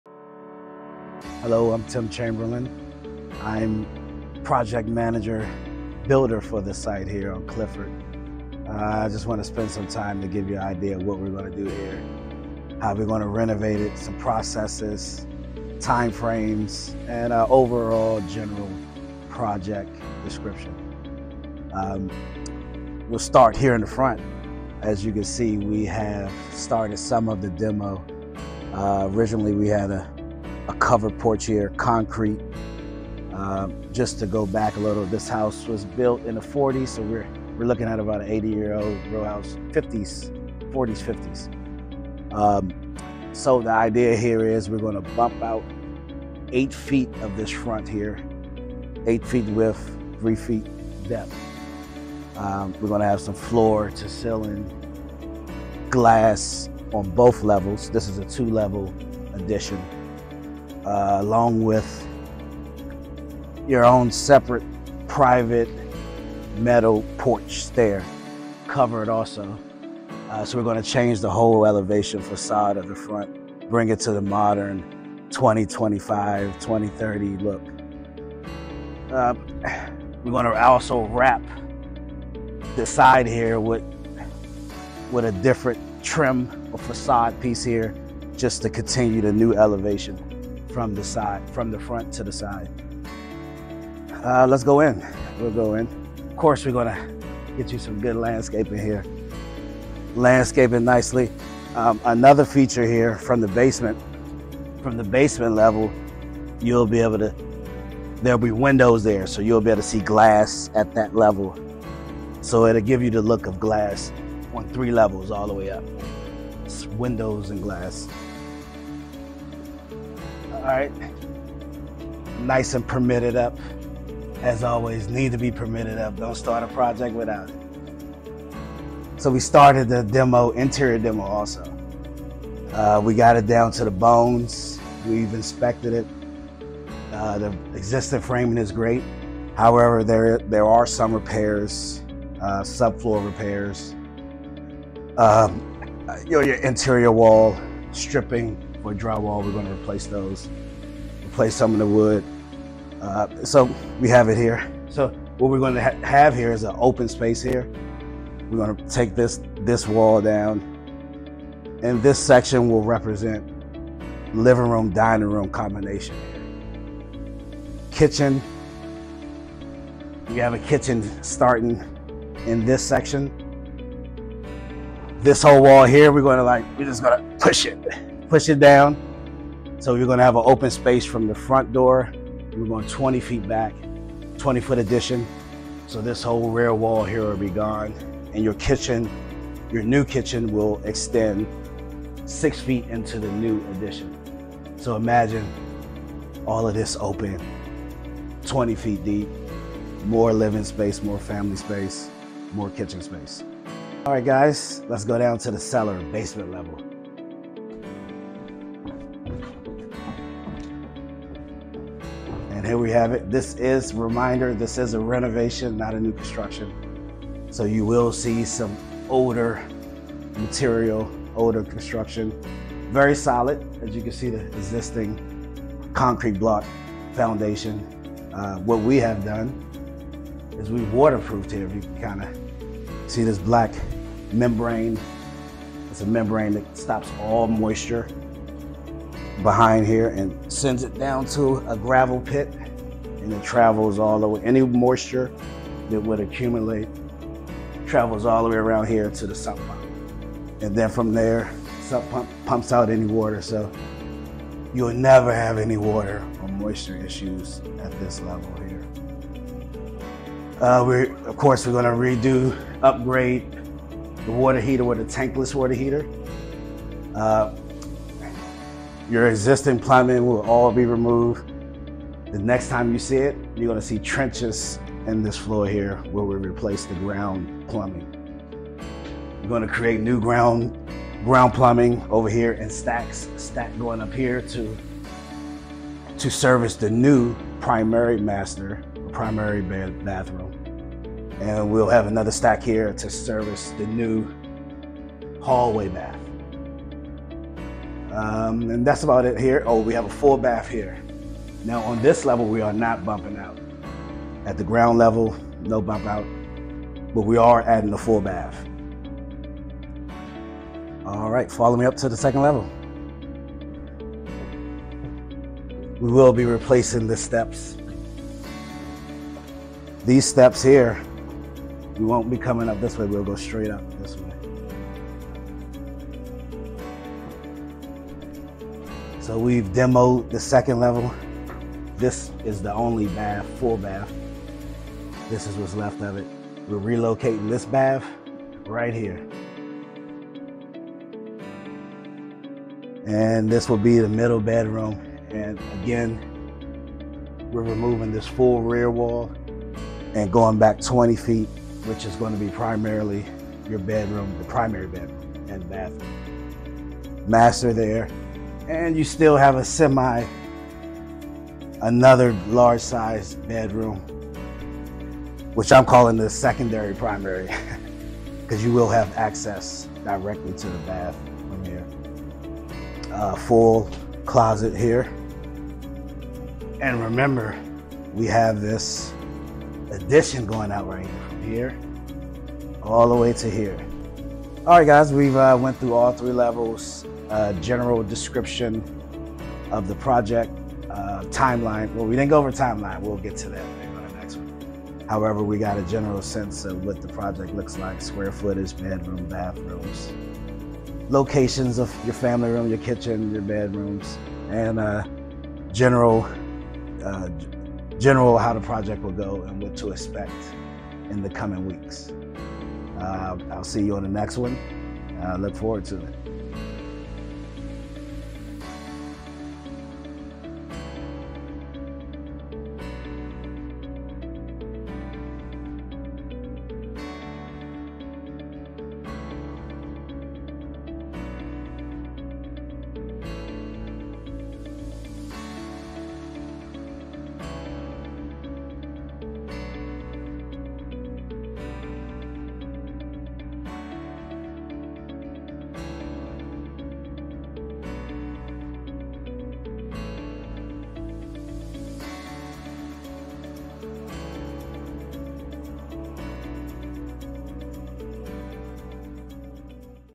Episode 5: Building the Vision
In Episode 5 of The Ultimate House Flip, the narrative shifts as Tim Chamberlain, the project’s trusted contractor and an experienced investor, takes the lead. As the mastermind behind the construction, Tim walks viewers through the major renovations that are shaping the future of the property.

He highlights key elements of the transformation, starting with the addition of new bathrooms and the dramatic 3-foot bump out in the front, designed to give the home more curb appeal. Tim also shares the exciting updates to the front façade, including the installation of additional windows, which are flooding the interior with natural light. At the back of the house, he discusses the impressive 20-foot extension, expanding the living space to create a more functional layout.

With meticulous attention to detail, Tim explains the critical improvements to the electrical and plumbing systems, ensuring the home is both safe and modernized. He also reveals the new layout for the primary bedroom, which has been relocated for better flow, while a second primary bedroom has been added to elevate the home’s value. And of course, the kitchen—a crucial part of any renovation—is being expanded to create a spacious, open-concept area perfect for entertaining.

At the end of the episode, Tim consults with John and India to provide them with an exciting update on the progress. The house is well on its way to becoming a beautiful, transformed space, and they’re right on schedule.

This episode is filled with both vision and execution, offering a detailed look at how careful planning and expert craftsmanship are bringing this project to life.